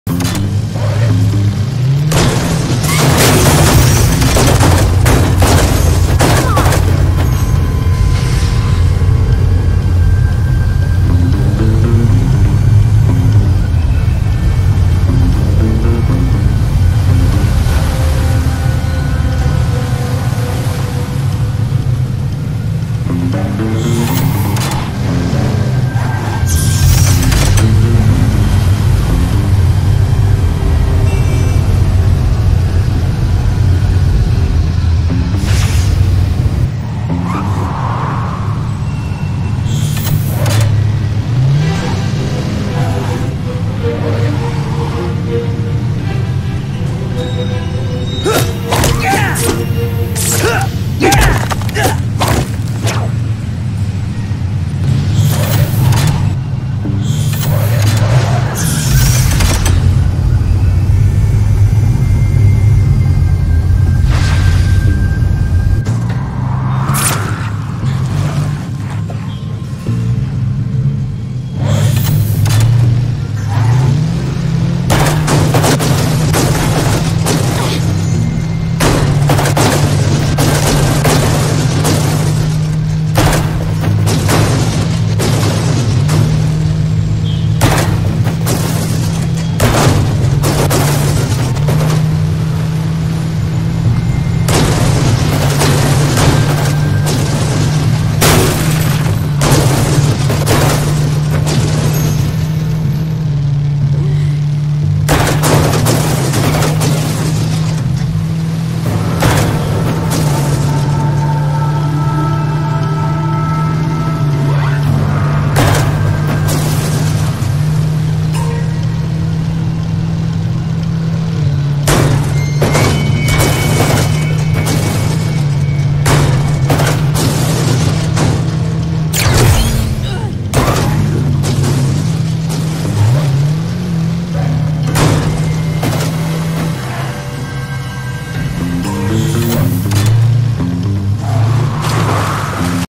Disney Spiderman 2 - Flying Cars
Welcome to part 2 of Disney Spiderman! Watch as spider-man drives around his awesome Shield super car and get around traffic by having the car fly!!! Be amazed at how his car can take off like a rocket ship!